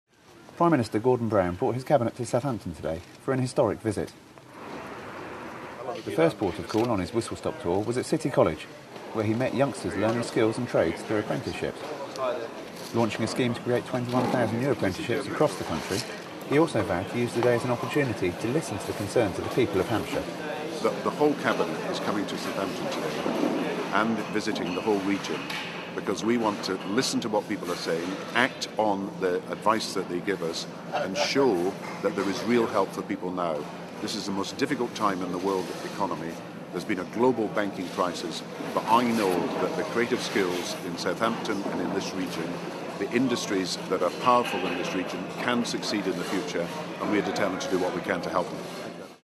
Prime Minister in Southampton
Prime Minister kicked off the Cabinet visit to Hampshire by meeting young apprentices at Southampton City College.